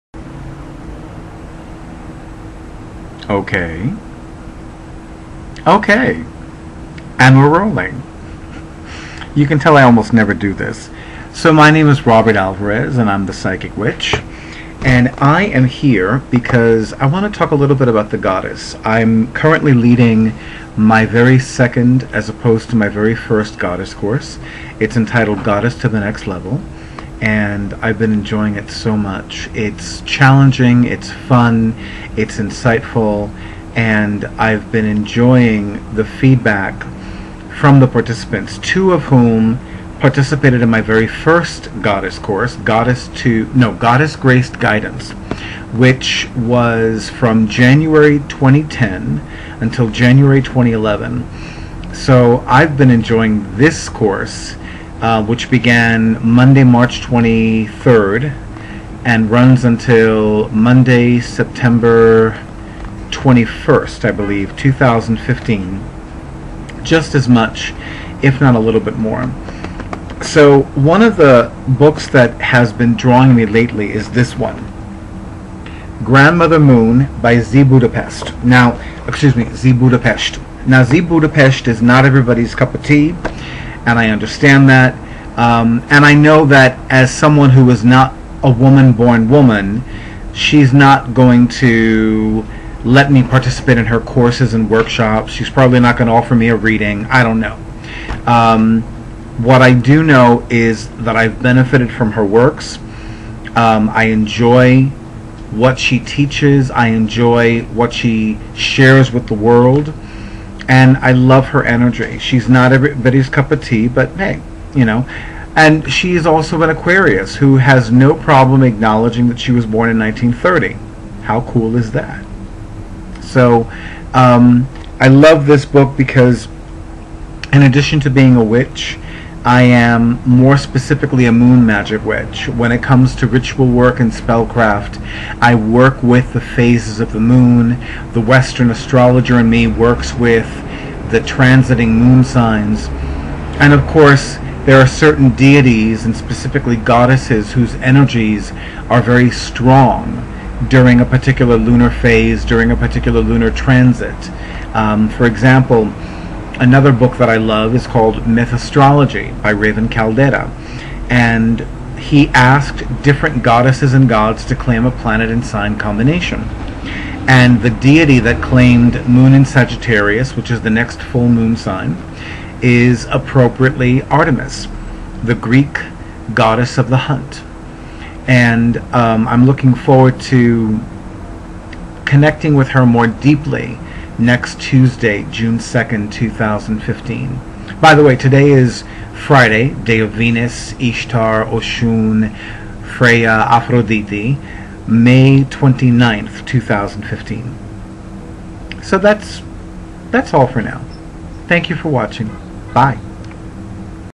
About the Goddess...
A short video featuring Robert Alvarez, the Psychic Witch, talking about his very second, as opposed to very first Goddess Course, "Goddess to the Next Level..." Z. Budapest, Moon Magick, and more!